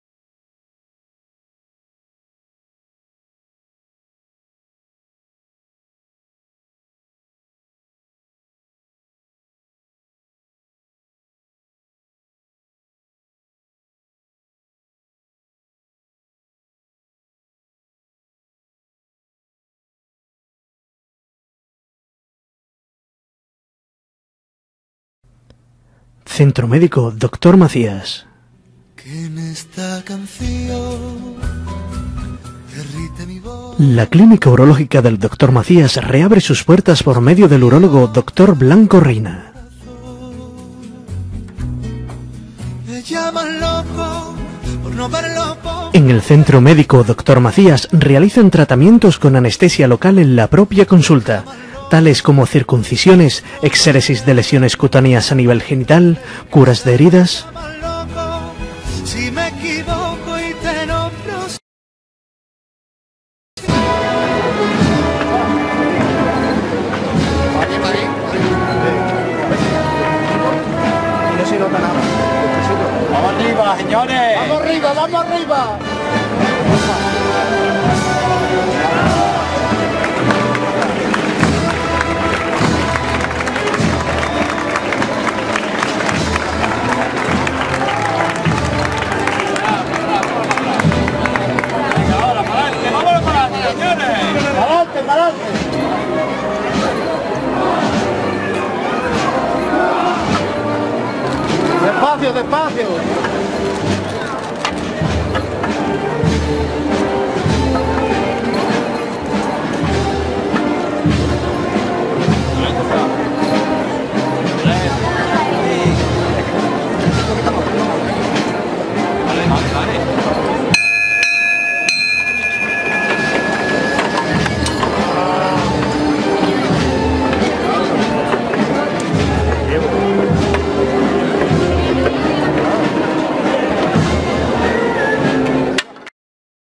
Encuentro Mayor Dolor en Alameda 12042017
ATQ Radio – TV graba para El Sol de Antequera, el encuentro de la Virgen y el Señor de la Cofradía del Mayor Dolor, en este Miércoles Santo en Antequera.
Las imágenes fueron grabadas el miércoles 12 de abril de 2017.